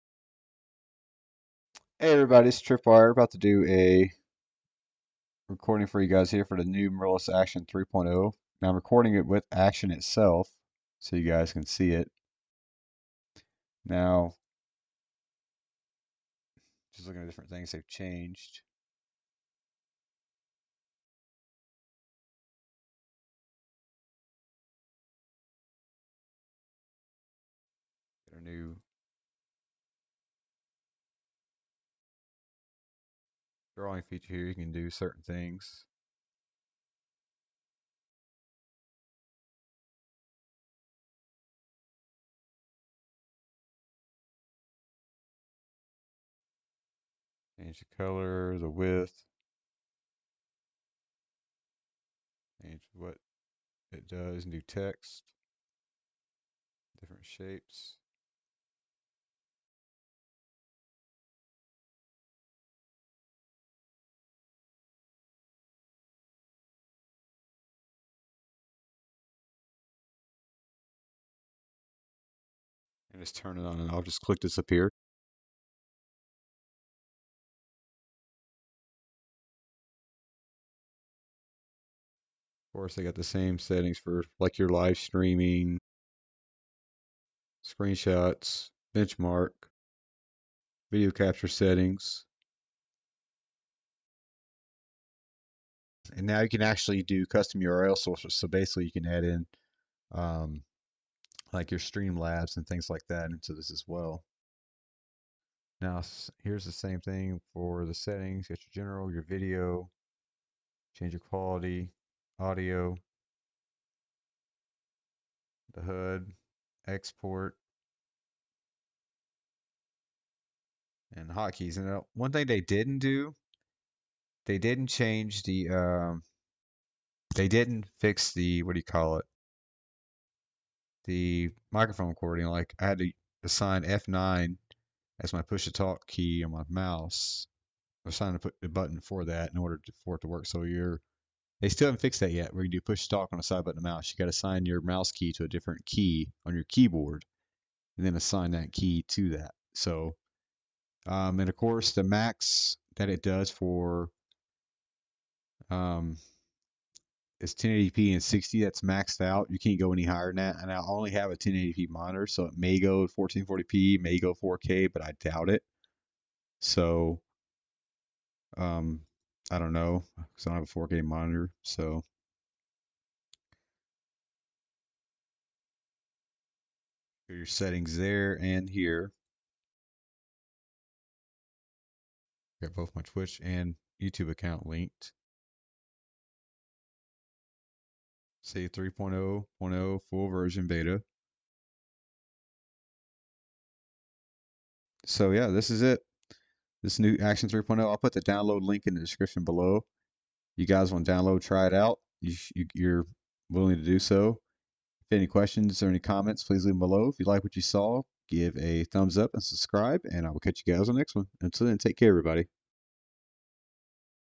Mirillis Action! 3.0.0 Beta | Updated Version Download link in description
To purchase Mirillis Action for full license key, buy it

This is a quick video I did over the new Mirillis Action 3.0 Beta that was released earlier today.  If you would like to download it the download link is

Changelog:
Hello everyone!

Check out the list below to find out what we have fixed!
Be sure to download the fixed version!

- Fixed support for extended mouse keys as hotkeys
- Added support for SSSE3, SSE4.1 and AVX CPU extensions (up to 2x performance improvement)
- Changed selected application recording mode for active screen selection (select desktop from dropdown list)
- Aux audio recording has been moved to audio settings tab
- Added function to split recordings into smaller parts during recording (when the system crashes most of the recording can be recovered). This function is available under video settings tab
- Max overlay/logo graphics size has been increased to 4K
- Improved Web overlay playback smoothness
- Added web overlay settings (set custom FPS)
- Fixed drawing panel on systems with more than 1 display connected
- Fixed webcam preview when recording AVI
- Improved performance for 4K recording
- Fixed problems with exporting/uploading window
- Added support for switching desktops on Windows 7 with Aero disabled
- Fixed recording of drawing panel on Windows 7 with Aero disabled
- Lots of small improvements, bug fixes and stability fixes